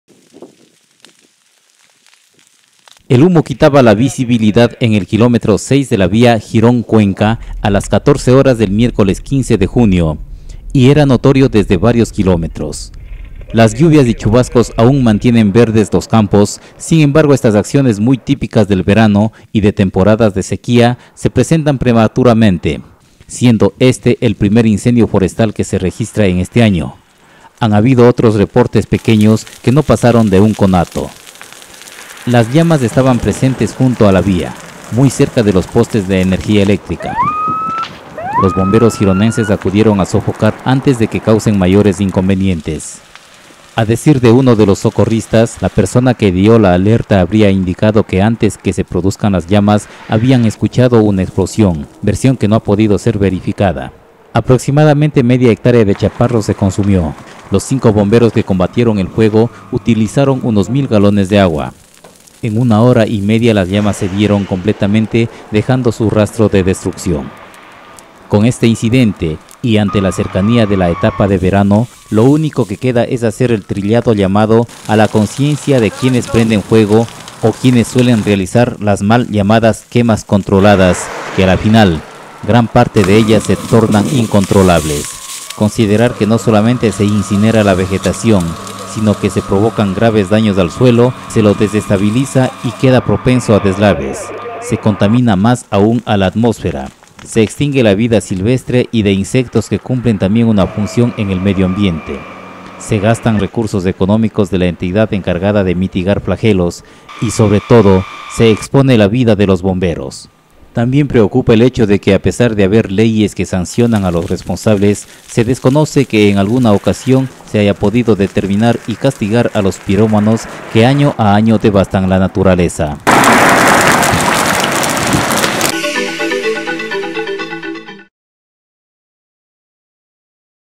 Aún no llega el verano y ya están los pirómanos
Hasta el momento tan sólo se habían registrado unos conatos; pero, el miércoles en la tarde se presentó el primer incendio forestal en la zona de Girón que abarcó una media hectárea de chaparros en el sector de El Masta, por estar junto a la vía, los bomberos pudieron  combatir con agua y en una hora y media las llamas cedieron.